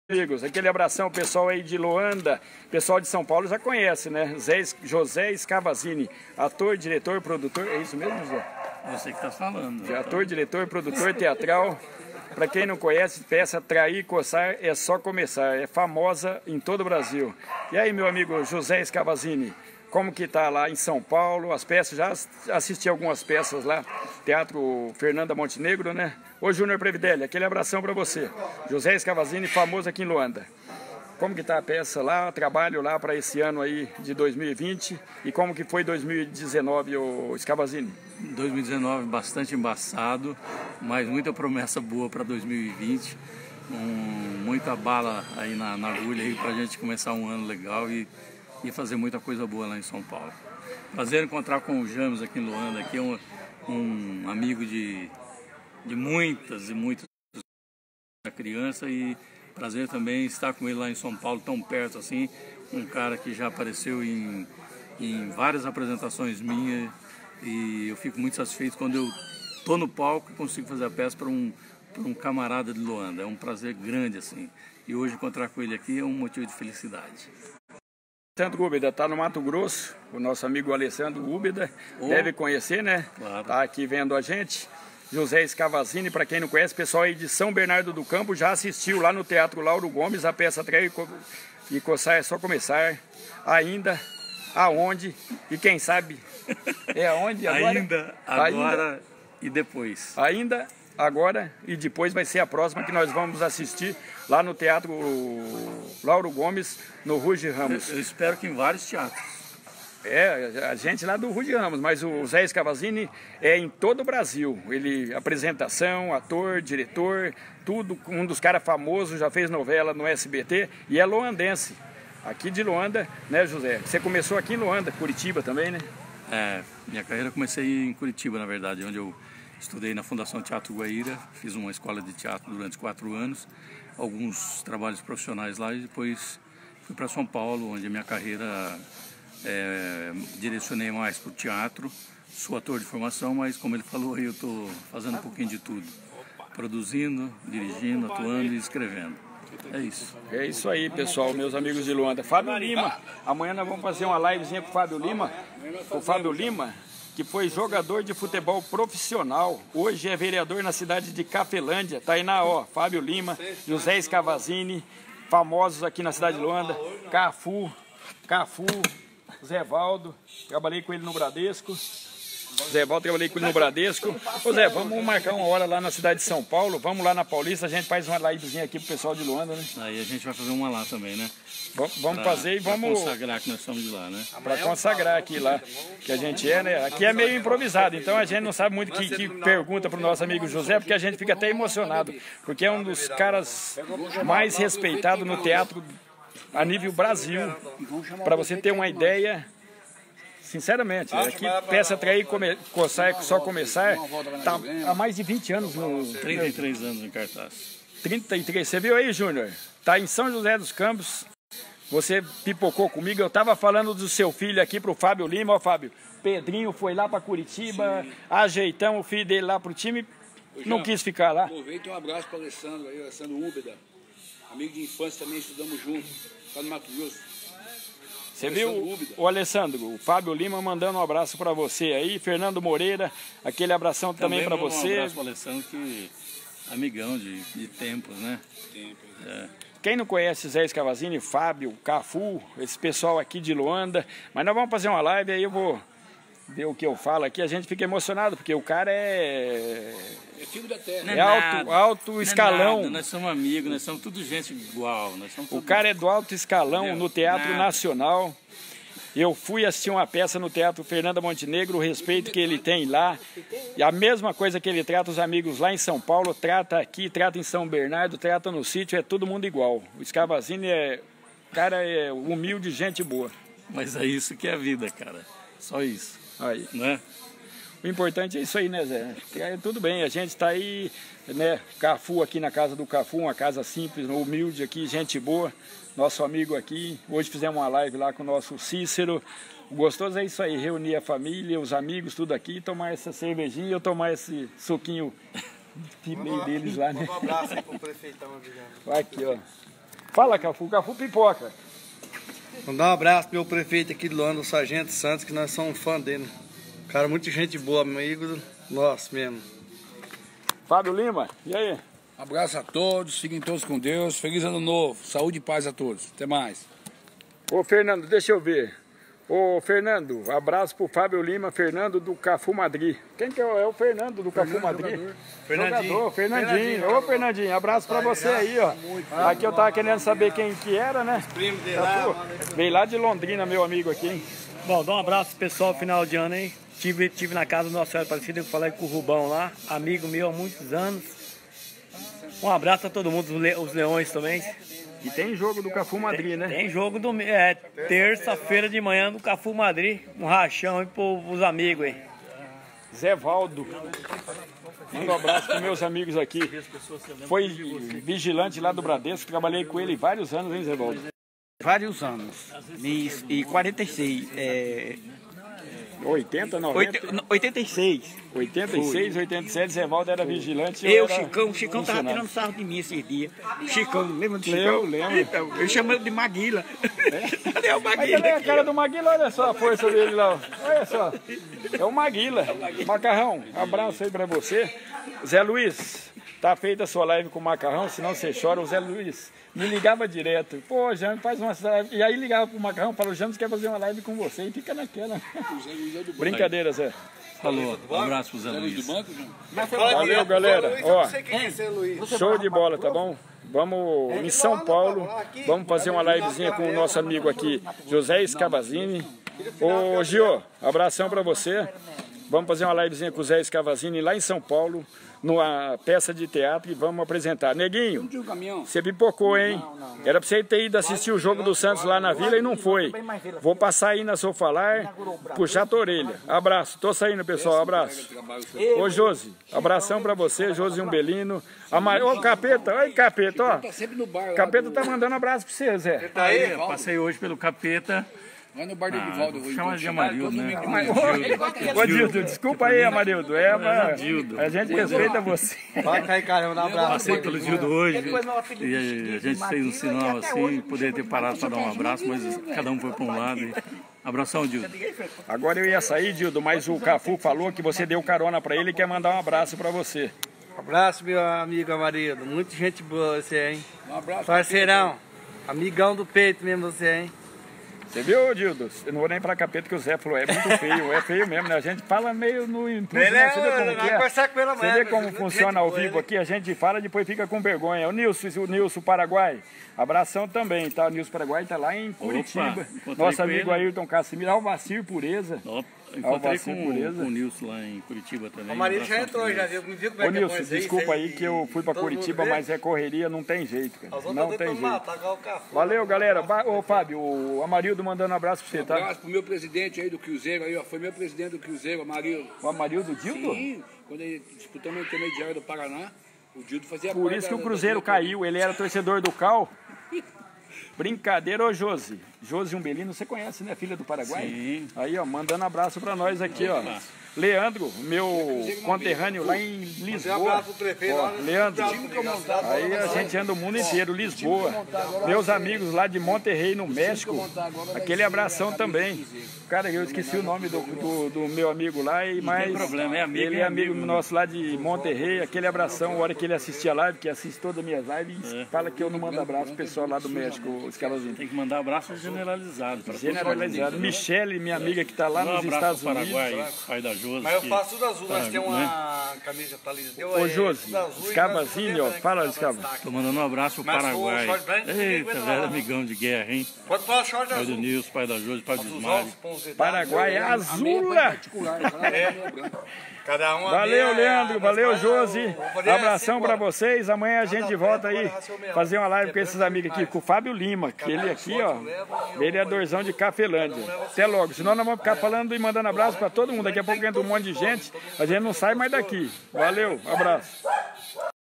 JOSÉ SCAVAZINI , Ator , Autor , Diretor e Produtor de Teatro , José Scavazini
James Rossi França conversando com o Loandense José Scavazini , Ator ,Autor , , Diretor  e Produtor Teatral.
Direção na peça ! Trair e coçar é só Começar , ator e autor em diversas peças de Teatro